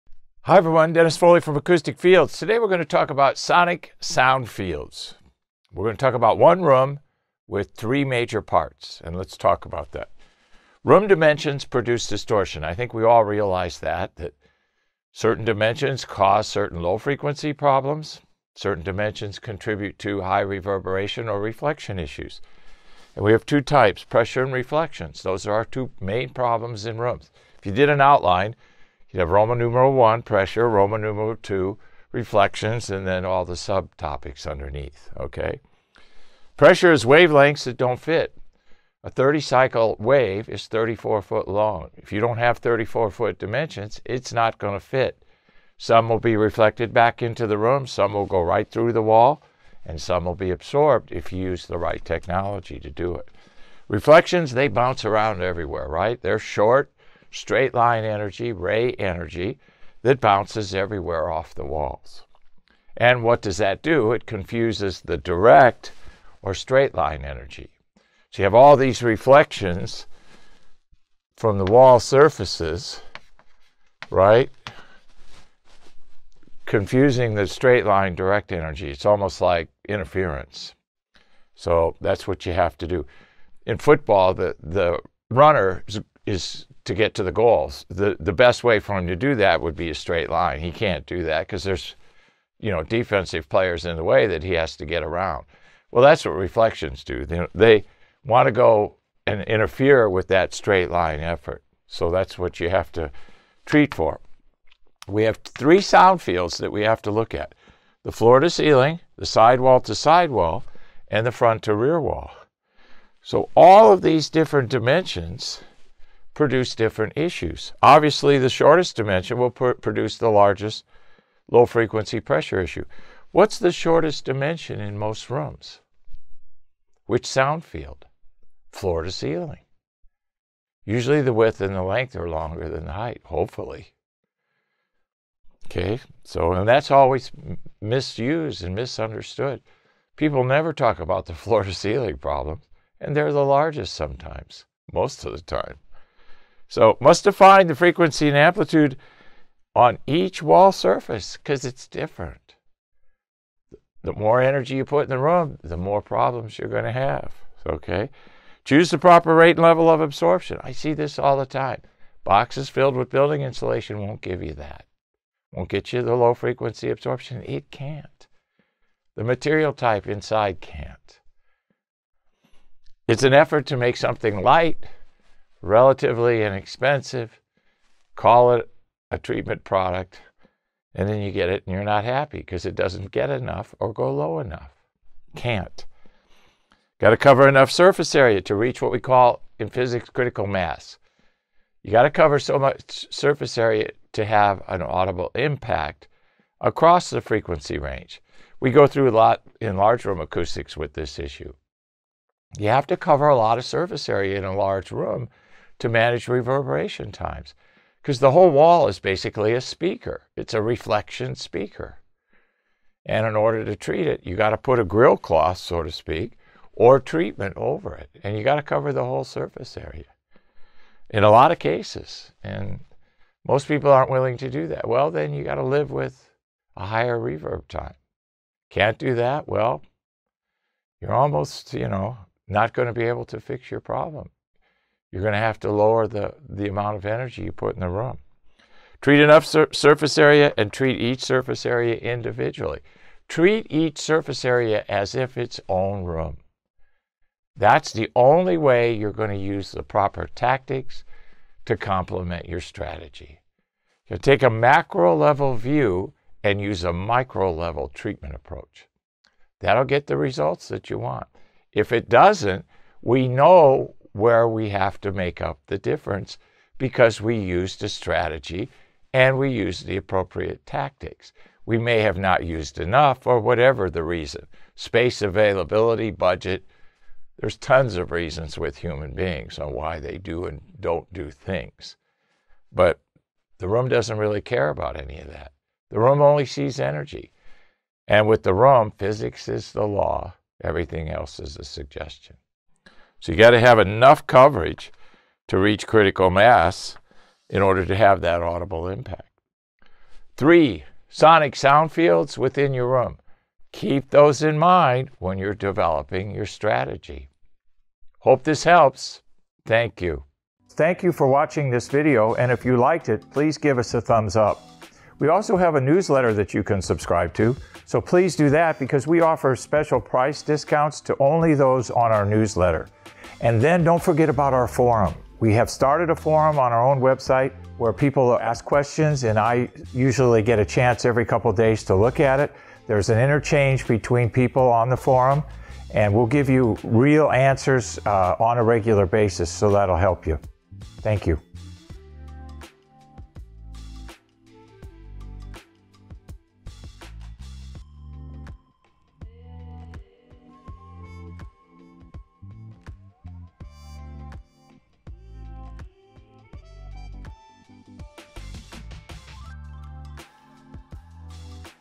Sonic Soundfields
- - In this video we're going to talk about the three sonic soundfields in our rooms. Watch the video to find out more!